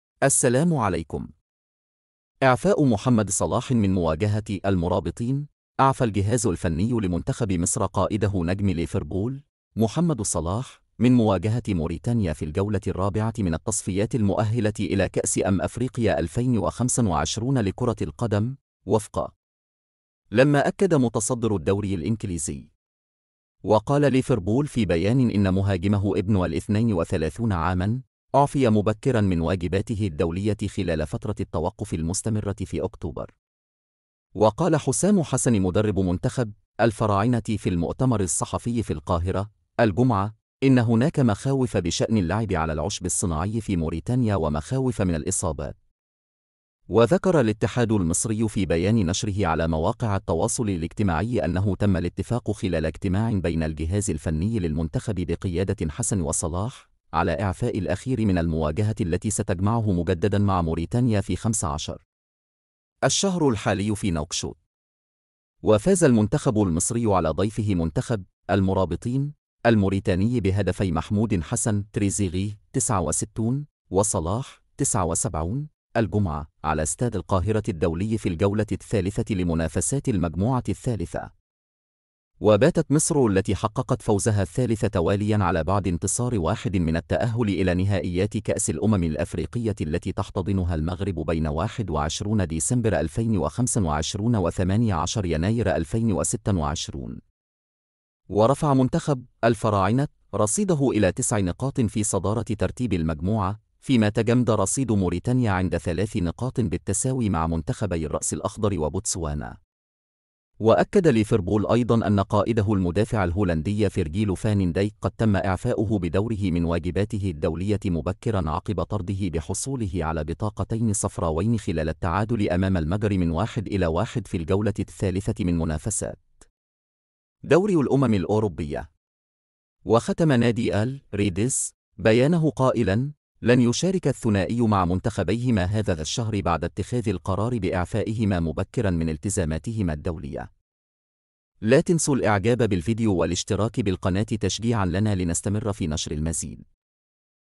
إعفاء محمد صلاح من مواجهة المرابطين
أعفى الجهاز الفني لمنتخب مصر قائده نجم ليفربول، محمد صلاح، من مواجهة موريتانيا في الجولة الرابعة من التصفيات المؤهلة إلى كأس أمم أفريقيا 2025 لكرة القدم، وفقا لما أكد متصدر الدوري الإنكليزي.

وقال ليفربول في بيان إن مهاجمه ابن الـ 32 عاما "أعفي مبكرا من واجباته الدولية خلال فترة التوقف المستمرة في أكتوبر".

وقال حسام حسن مدرب منتخب "الفراعنة" في المؤتمر الصحفي في القاهرة، الجمعة، إن هناك مخاوف بشأن اللعب على العشب الصناعي في موريتانيا ومخاوف من الإصابات.

وذكر الاتحاد المصري في بيان نشره على مواقع التواصل الاجتماعي أنه تم الاتفاق خلال اجتماع بين الجهاز الفني للمنتخب بقيادة حسن وصلاح، على إعفاء الأخير من المواجهة التي ستجمعه مجددا مع موريتانيا في 15 الشهر الحالي في نواكشوط.

وفاز المنتخب المصري على ضيفه منتخب "المرابطين" الموريتاني بهدفي محمود حسن "تريزيغيه"
(69)وصلاح (79)، الجمعة، على ستاد القاهرة الدولي في الجولة الثالثة لمنافسات المجموعة الثالثة.

وباتت مصر التي حققت فوزها الثالث تواليا على بُعد انتصار واحد من التأهل إلى نهائيات كأس الأمم الأفريقية التي تحتضنها المغرب بين 21 ديسمبر 2025 و18 يناير 2026.

ورفع منتخب "الفراعنة" رصيده إلى تسع نقاط في صدارة ترتيب المجموعة، فيما تجمّد رصيد موريتانيا عند ثلاث نقاط بالتساوي مع منتخبي الرأس الأخضر وبوتسوانا.
وأكد ليفربول أيضا أن قائده المدافع الهولندي فيرجيل فان دايك قد تم إعفاؤه بدوره من واجباته الدولية مبكرا عقب طرده بحصوله على بطاقتين صفراوين خلال التعادل أمام المجر 1-1 في الجولة الثالثة من منافسات دوري الأمم الأوروبية.

وختم نادي الـ"ريدز" بيانه قائلا "لن يشارك الثنائي مع منتخبيهما هذا الشهر بعد اتخاذ القرار بإعفائهما مبكرا من التزاماتهما الدولية".